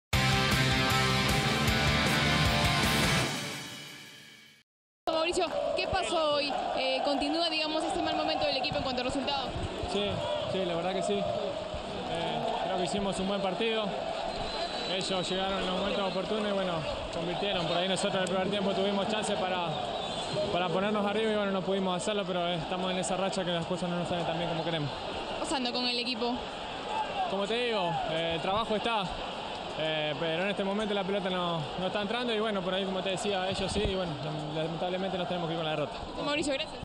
Mauricio Affonso sobre el clásico: "En este momento la pelota no está entrando"
Alianza Lima sufrió una dolorosa derrota esta noche con Universitario en Matute y los jugadores no la pasan bien. Las declaraciones de Mauricio Affonso luego del clásico dejaron en claro que las cosas no están saliendo como trabajan en la semana y eso pone mucho más difícil este momento que viven en la Liga1 Movistar.